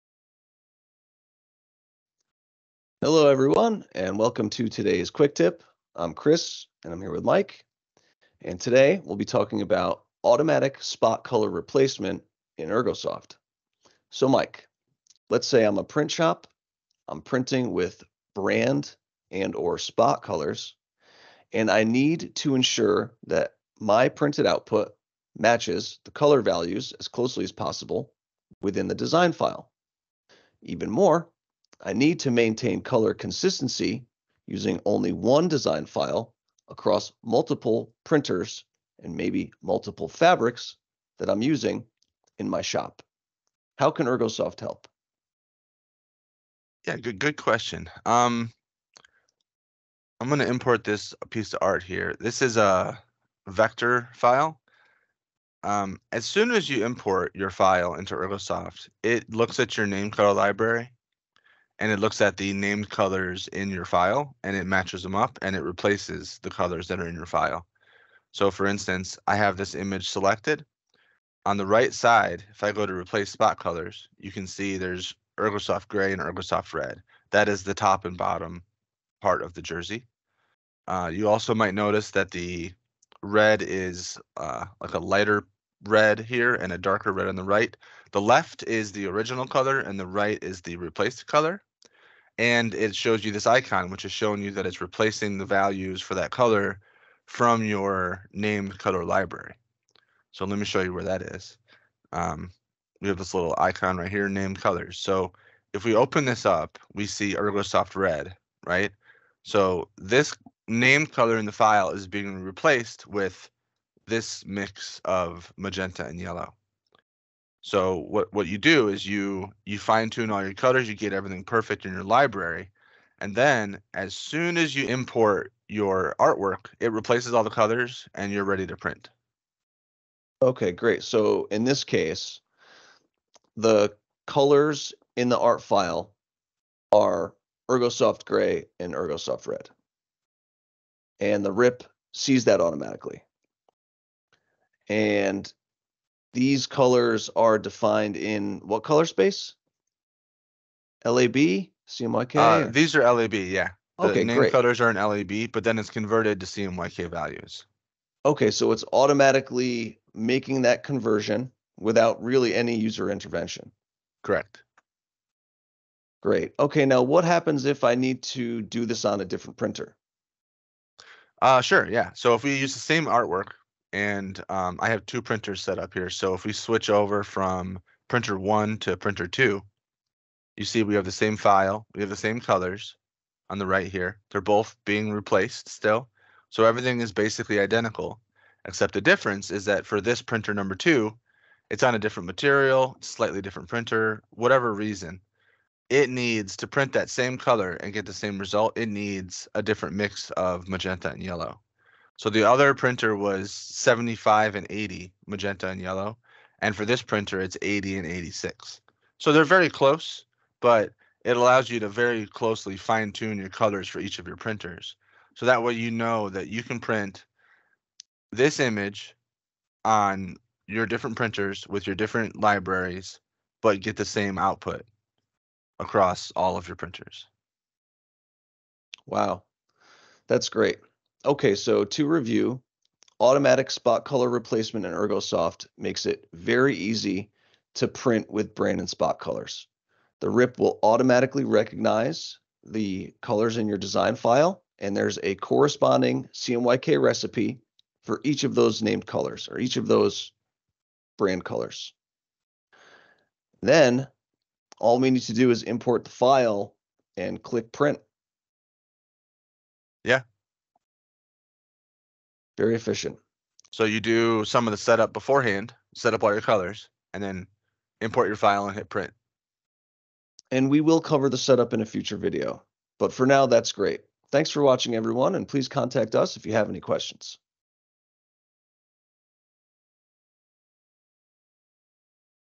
Color Replacement - Quick Tip 1 - Auto Spot Color Replacement
Welcome to the Quick Tip series about color replacement with Ergosoft RIP. This video teaches you how to set up automatic spot color replacement.